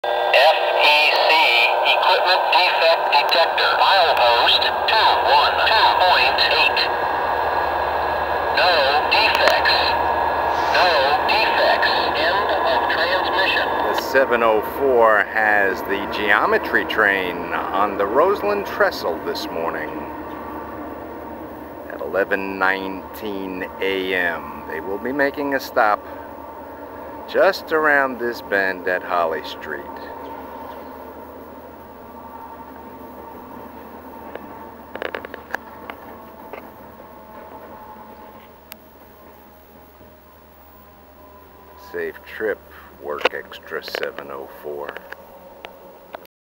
WORK704 Oct 11, 2012
Geometry train across the Roseland Trestle on a passenger schedule again today, will be making a stop for guests at Holly Street shortly.
EoT past DD MP212.8 NO Defects at 11:19am.